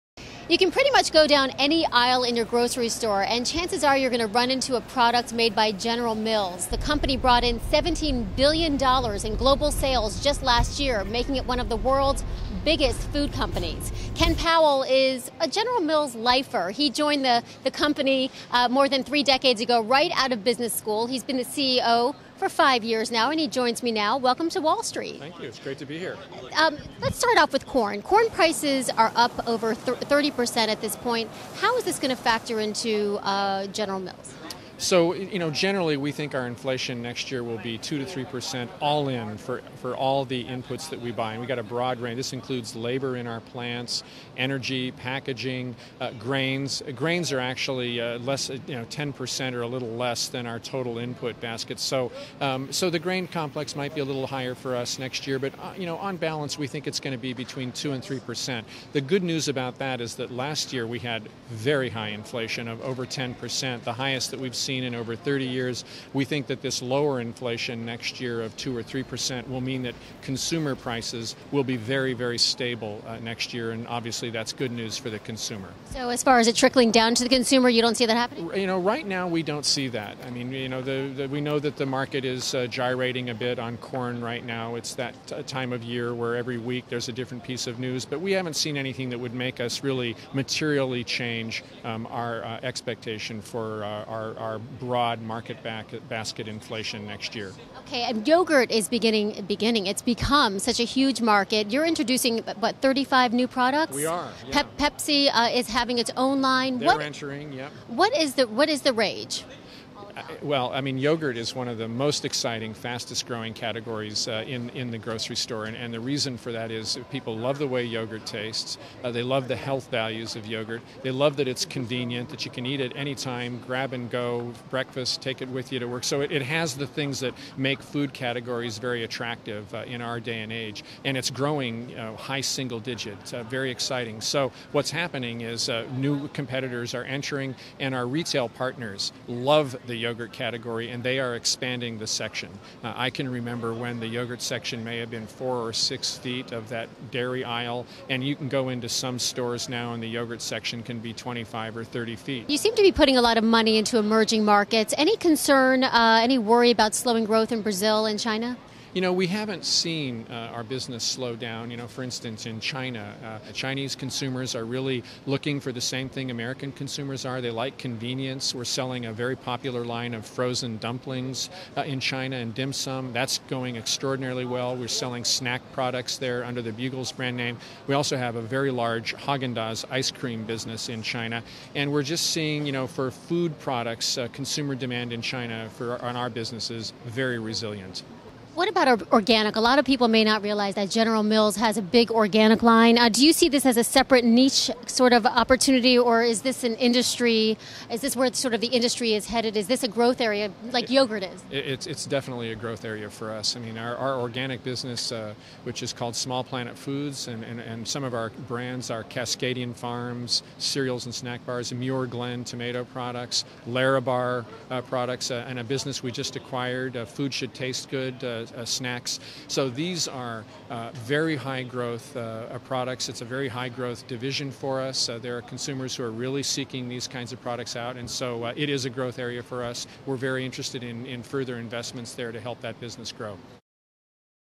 Why corn price spike won't hit your wallet
General Mills CEO says input costs for his company's products will go up only 2-3% over the next year compared to 10% last year.